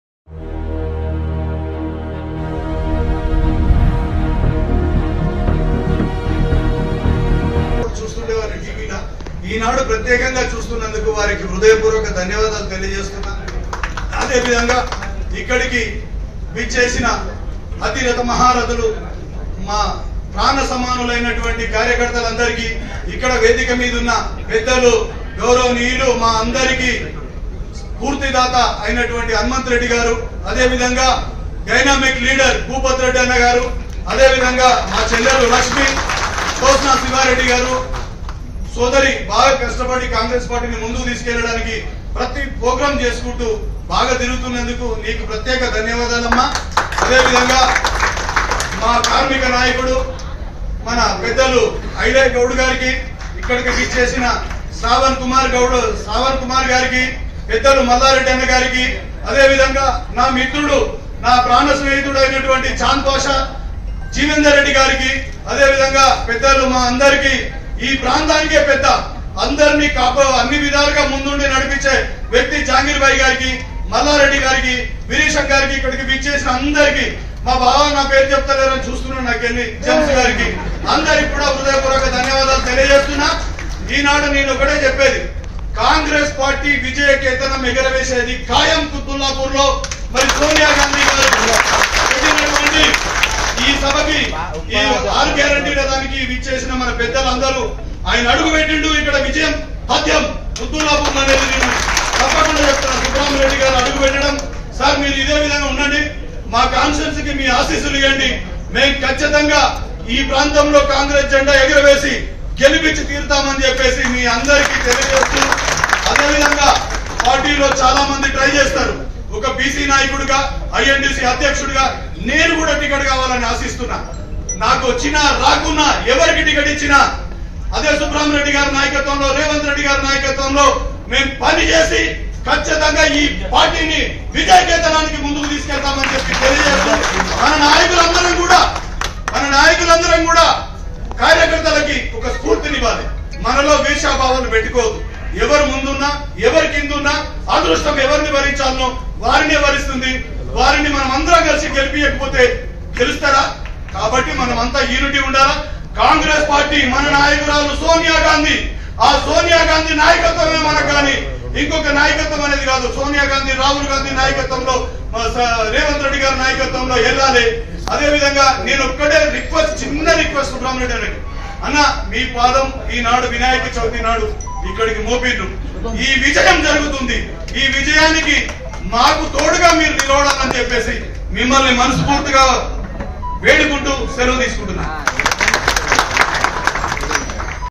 Quthbullapur : Congress Leader Gunja Srinivas Speech Infront Ex MP Subbarami Reddy | NEWS16 Telugu
/ news16telugu​.  .